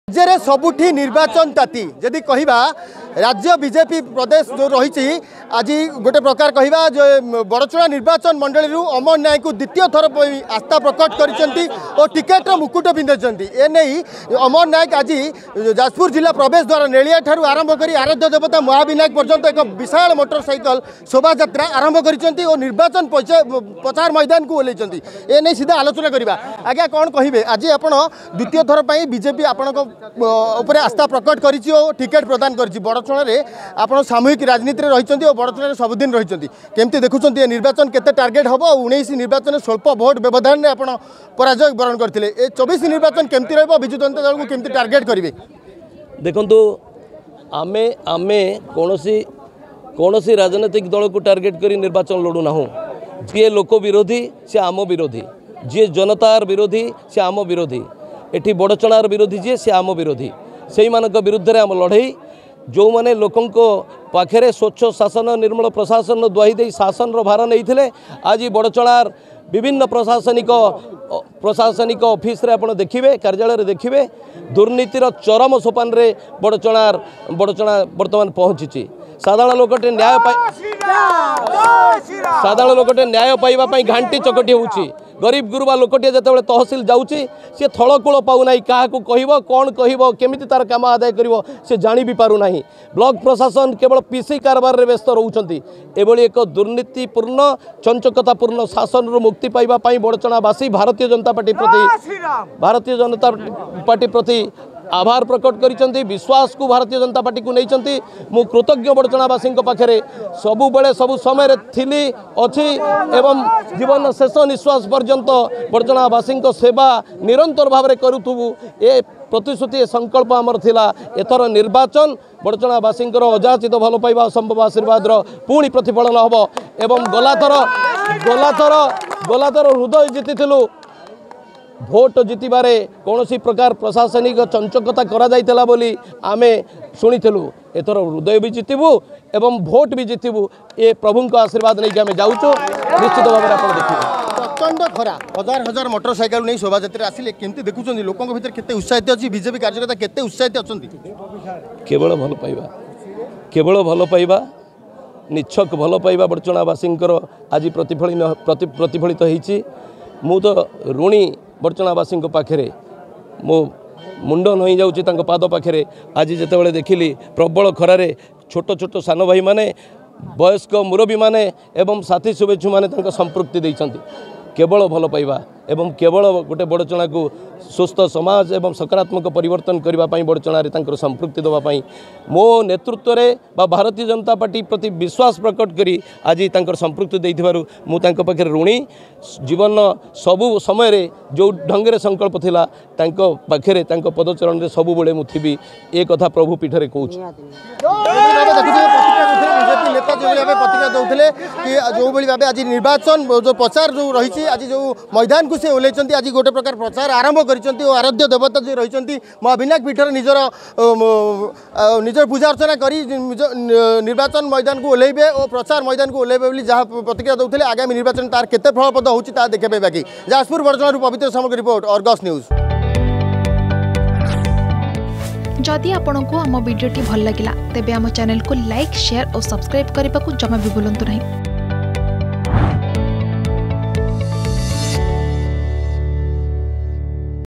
ପୂଜାର୍ଚ୍ଚନା ସାରି ପ୍ରଚାର ମଙ୍ଗ ଧରିଲେ ବିଜେପି ପାର୍ଥି ଅମର ନାୟକ || Amar Nayak || BJP
ଯାଜପୁର ବଡ଼ଚଣାରୁ ଅମର ନାୟକଙ୍କୁ ବିଜେପି ଟିକେଟ ମିଳିବା ପରେ, ହଜାର ହଜାର କର୍ମୀଙ୍କ ସହ ଚଣ୍ଡିଖୋଲ ସ୍ଥିତ ମହାବିନାୟକ ପୀଠରୁ ପୂଜାଚର୍ନା କରି ପ୍ରଚାର ମଙ୍ଗ ଧରିଛନ୍ତି।